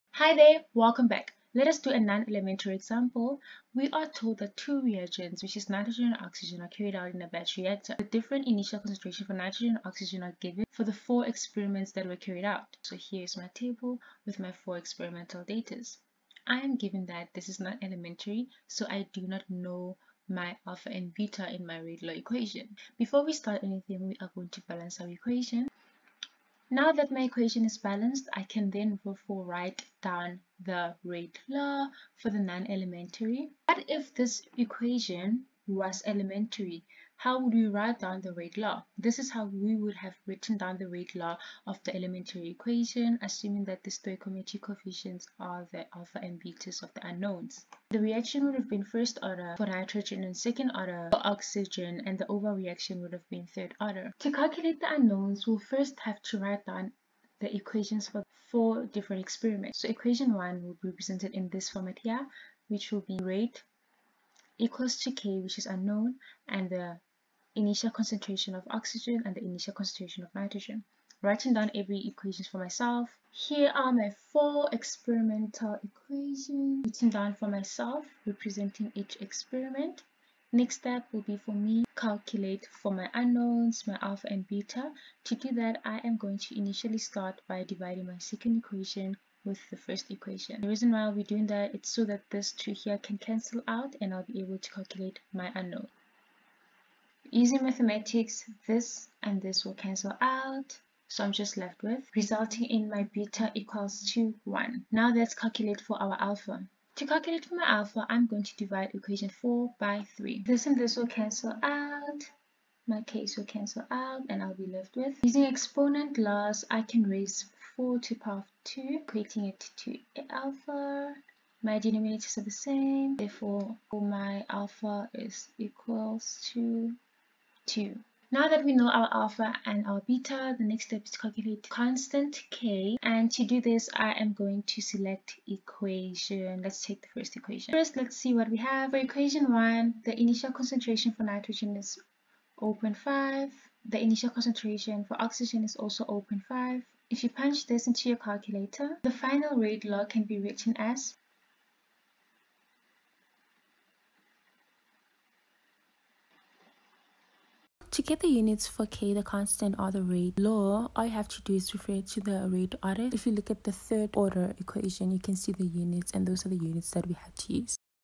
𝑁2 + 2𝑂2 → 2𝑁𝑂2: Calculate the Rate Law (Non elementary): 𝒓𝒂𝒕𝒆 𝒍𝒂𝒘 = 𝒓 = 𝒌 [𝑵]∝ [𝑶𝟐]𝜷
Determine / calculate the Rate Law (Non elementary): 𝒓𝒂𝒕𝒆 𝒍𝒂𝒘 = 𝒓 = 𝒌 [𝑵]∝ [𝑶𝟐]𝜷

Two reactions between compounds N and O were carried out in a batch reactor. The reaction rate for different N and O starting concentrations were determined as shown.

𝑁2 + 2𝑂2 → 2𝑁𝑂2 (Single step reaction)
Exp             Initial concentrations                    Initial rate (r)
  [𝑁2] / 𝑚𝑜𝑙. 𝑑𝑚−3    [𝑂2] / 𝑚𝑜𝑙. 𝑑𝑚−3     / 𝑚𝑜𝑙. 𝑑𝑚−3.𝑠−1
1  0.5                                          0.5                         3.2 𝑥 10−3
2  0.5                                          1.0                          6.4 𝑥 10−3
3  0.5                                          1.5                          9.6 𝑥 10−3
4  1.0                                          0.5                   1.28 𝑥 10−2

Non elementary, stoichiometry of the reaction order of each species is not identical to the stoichiometric coefficient for the species for the reaction.
𝑟𝑎𝑡𝑒 𝑙𝑎𝑤 = 𝑟 = 𝑘 [N2] ∝ [O2]
𝑒𝑞𝑢𝑎𝑡𝑖𝑜𝑛 1:      3.2 𝑥 10−3 = (0.5)∝(0.5)𝛽
𝑒𝑞𝑢𝑎𝑡𝑖𝑜𝑛 2:      6.4 𝑥 10−3 = (0.5)∝(1.0)𝛽
𝑒𝑞𝑢𝑎𝑡𝑖𝑜𝑛 3:      9.6 𝑥 10−3 = (0.5)∝(1.5)𝛽
𝑒𝑞𝑢𝑎𝑡𝑖𝑜𝑛 4:      1.28 𝑥 10−2 = (1)∝(0.5)𝛽